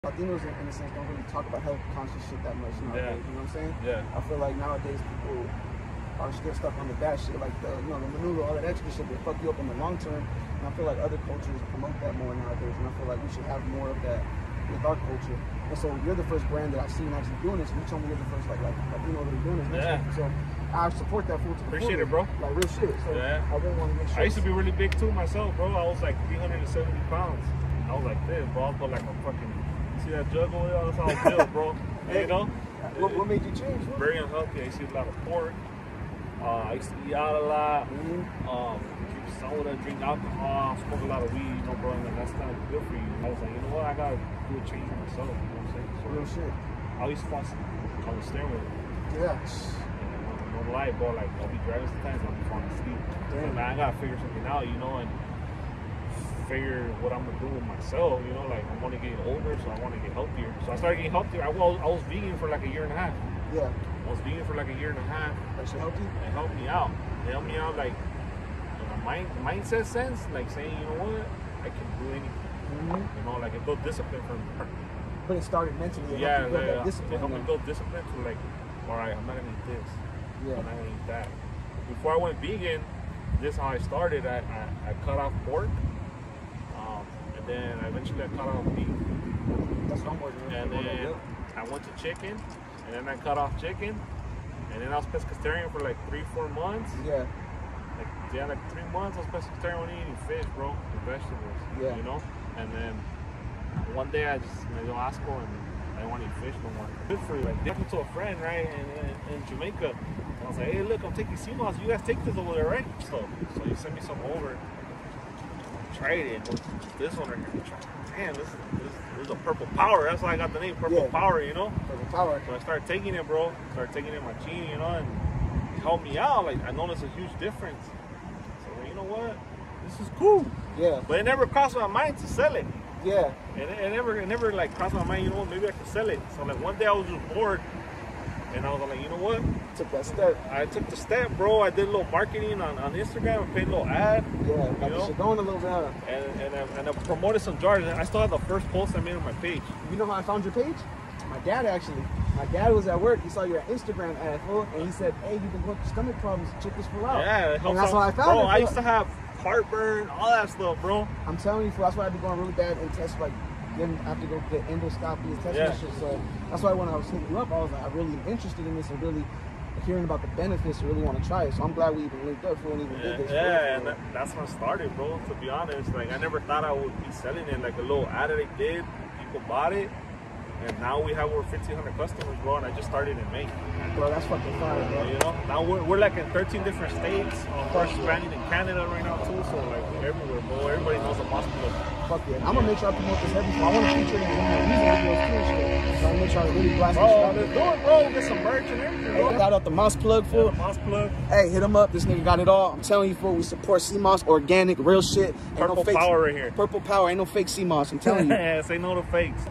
ELDAHRADO Link’s up with @Da_Moss_Plug To Speak On SeaMoss Benefits PT.1
I link up with @Da_Moss_Plug to talk about what inspired him to create his seamoss company and the health issues that led him change his lifestyle/diet